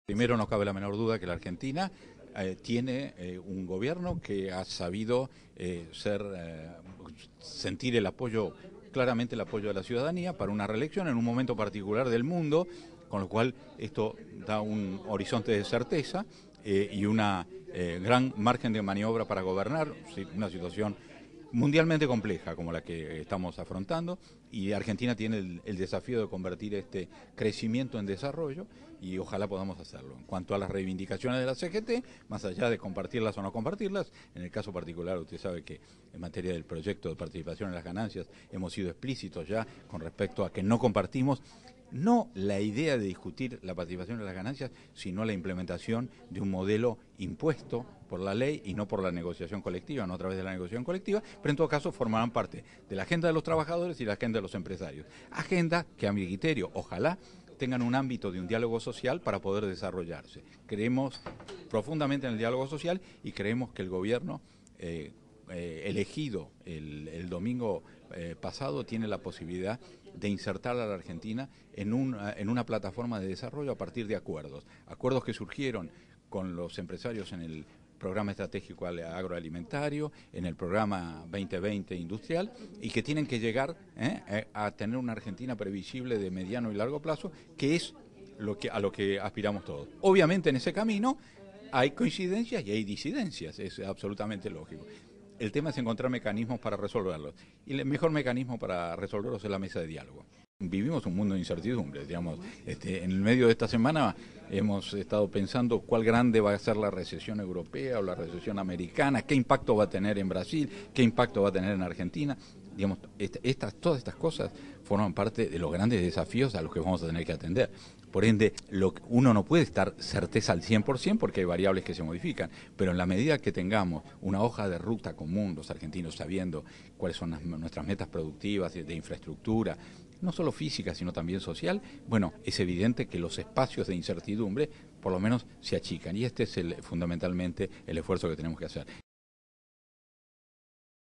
La UIA quiere que la participación en las ganancias surja de una negociación colectiva.
El vicepresidente de la Unión Industrial Argentina, Daniel Funes de Rioja, advirtió que la  participación de los trabajadores en las ganancias debe surgir de una negociación colectiva y no a partir de una ley.
"No compartimos la implementación de un modelo impuesto por la ley y no por la negociación colectiva", expresó Funes de Rioja.
Por otra parte, el directivo valoró la reelección de Cristina Fernández de Kirchner y consideró que "Argentina tiene el desafío de convertir el crecimiento en desarrollo".
Al referirse sobre un eventual impacto en el país de la crisis internacional, Funes de Rioja sostuvo que "en la medida en que los argentinos tengamos una hoja de ruta en común, los espacios de incertidumbre se achicarán".